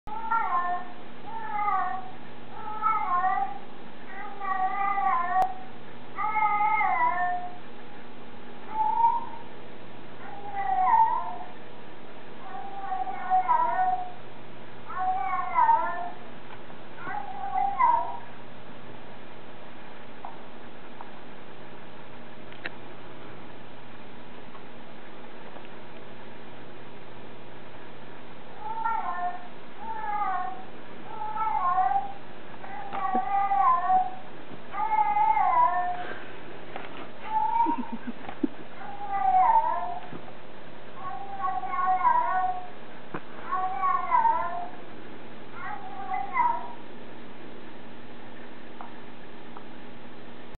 Isis responds to Tiggy's talking
Black bobtail trying to find the talking cat.